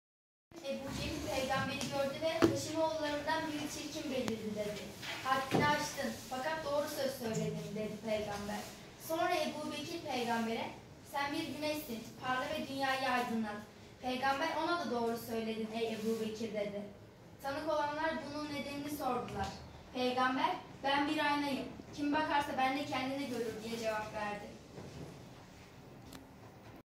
Zit Gorusler
Zit gorusler adli hikayeyi okuduk.